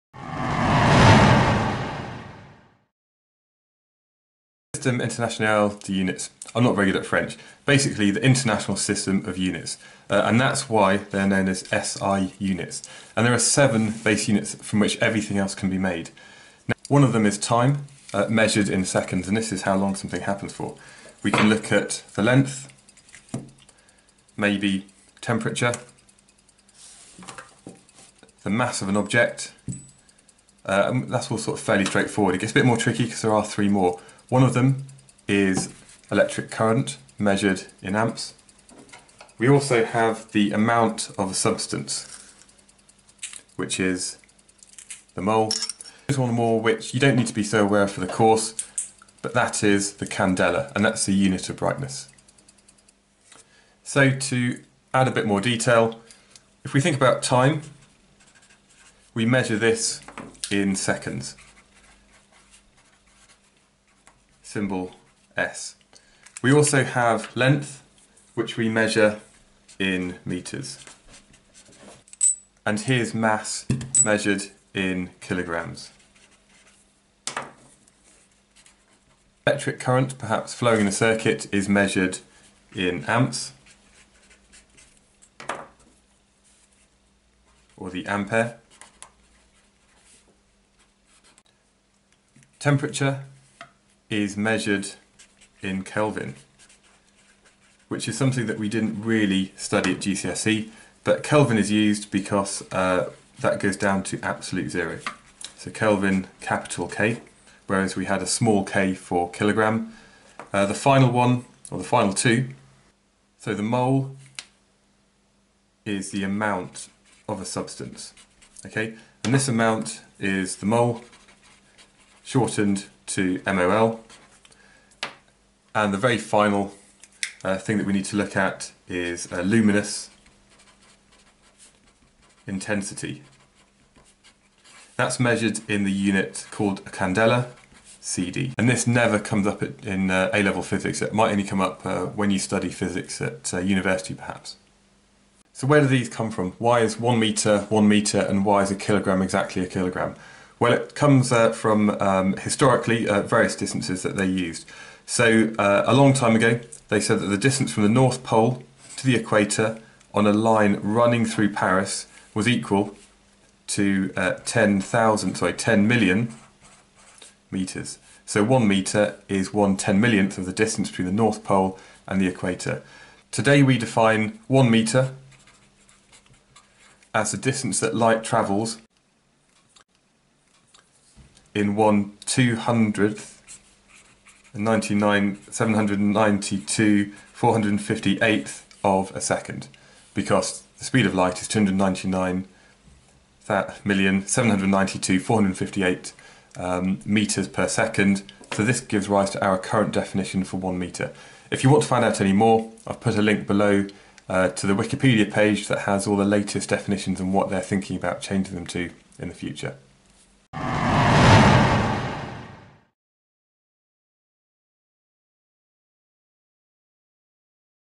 Base Units - A Level Physics
This video introduces and explains base units for A Level Physics.

Base units are otherwise known as SI units (Systeme International d'Unites). The seven base units or SI units are: kilograms, seconds, metres, kelvin, amperes, candelas and moles.

To find more about the exact definitions have a look at Wikipedia

Lewis

AQA
CIE
Edexcel
Edexcel IAL
Eduqas
IB
OCR A
OCR B
WJEC